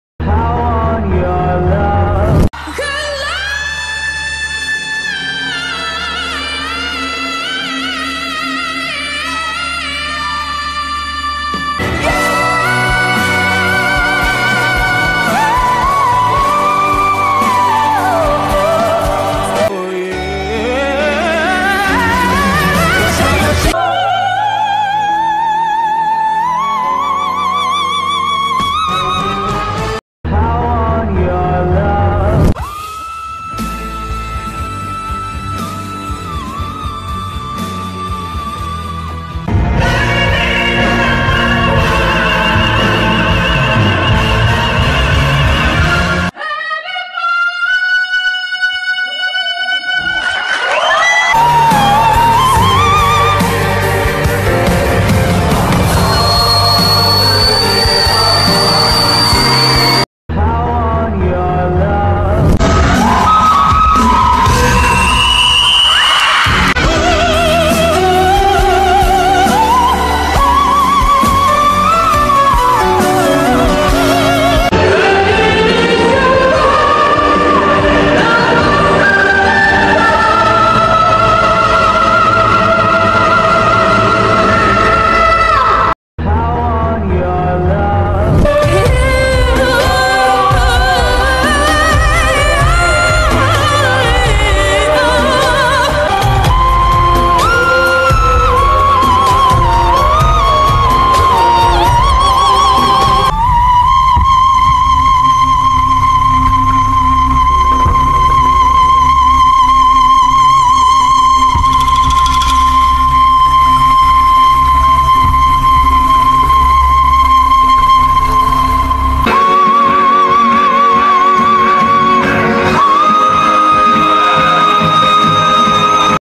Димаш Кудайберген и его нереальные ноты 🎶
Я - поклонница Димаша.

King of high notes👑bu Part 1'i, keşfete düşene kadar Part 2'sini 3'ünü de yapmayı planlıyorum, malum daha efsanevi D8'i ve daha nice muhteşem performansları koyamadım, reels süreleri sağ olsun:')

Источник Instagram.com/dimashmusicturkey